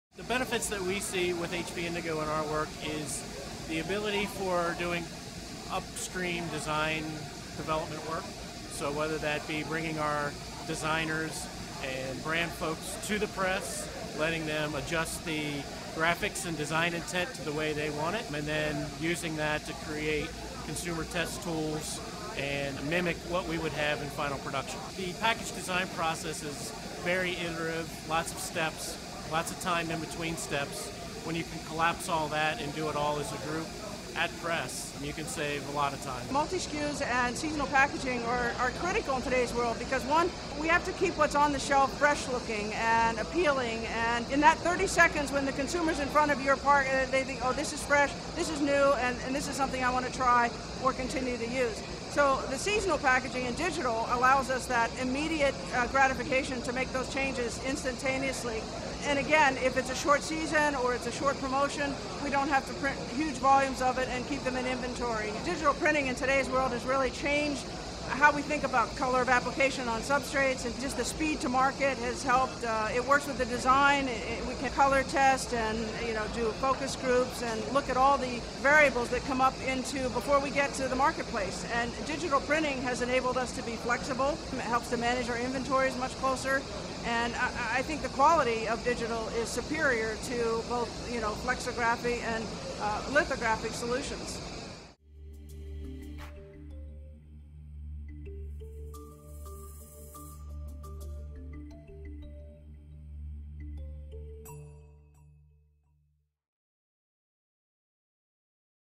Brand managers speak out on digital package design and production
On April 27, CPC brand owners, designers, production and procurement professionals learned about full-scale digital manufacturing for PS labels, flexible packaging and more at the Follow Possible Package Printing Forum presented by HP and hosted by Innovative Labeling Solutions (ILS) in Hamilton, Ohio. Labels & Labeling offers this exclusive video interview with Dan Theiss, Procter & Gamble and Jaye Johnson, Artico.

Former Procter & Gamble executive and current CPG consultant Mike Ferrari spoke about CPGs' packaging needs and the ways digital printing can address them. The event featured ILS's three HP Indigo WS6000 production lines producing pressure sensitive labels, shrink sleeves, flexible packaging and other products at full-color speeds up to 98 feet per minute.

The presses were shown in complete in-line and near-line finishing workflows, including the first-ever public display of a Delta Industrial finishing solution installed in-line with an HP Indigo press.